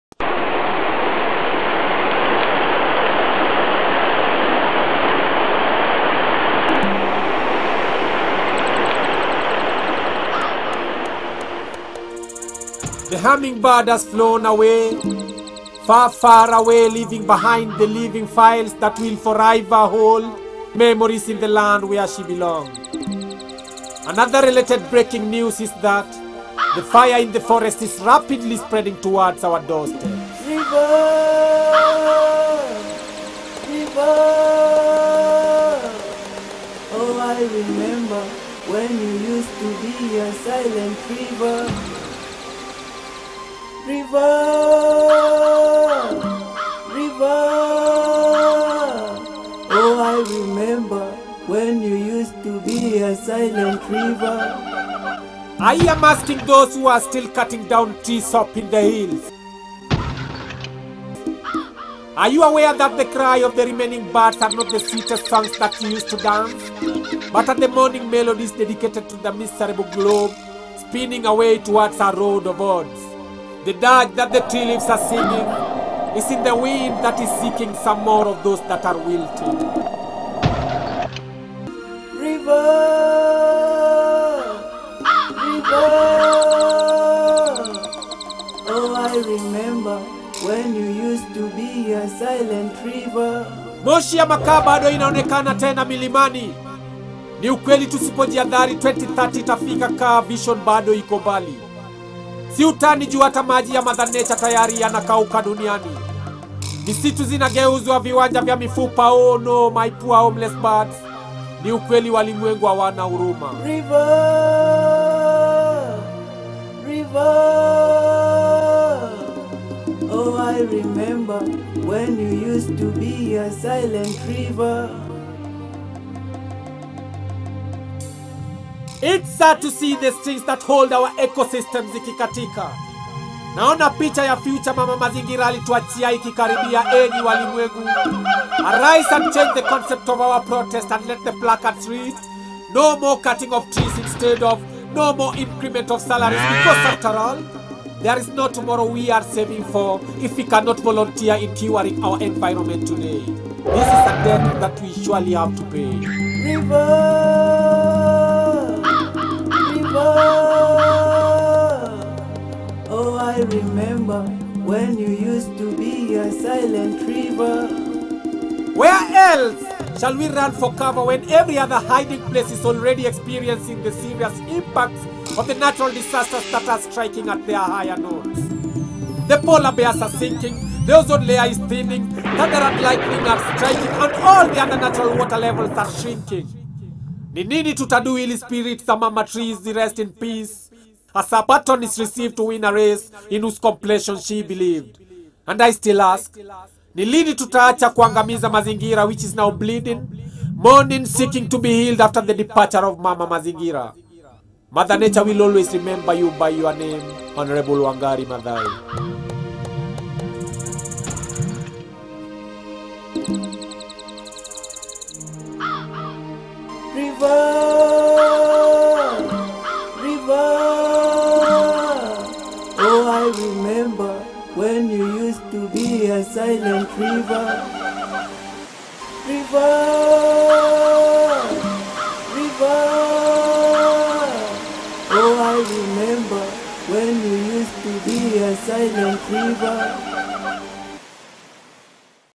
SILENT RIVER (In memory of Prof. Wangari Maathai
She faced a lot in her fight to wipe away Mama Earth's tears but here we are again, doing the worst that she did not see: Burning our own home.